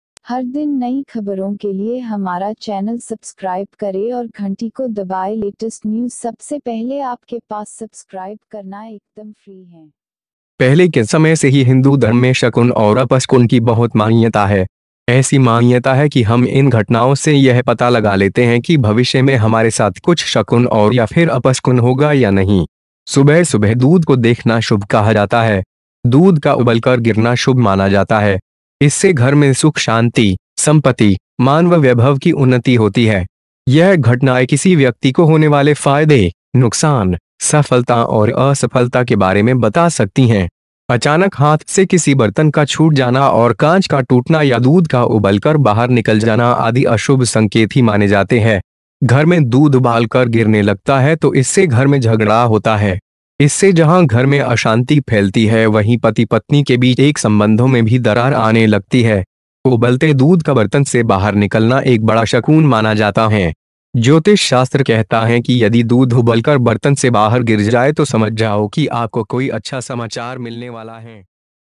What happens when milk falls out of boiling utensils, auspicious or inauspicious
क्या होता जब दूध उबलकर बर्तन से बाहर गिर जाए शुभ या अशुभ देखे वीडियो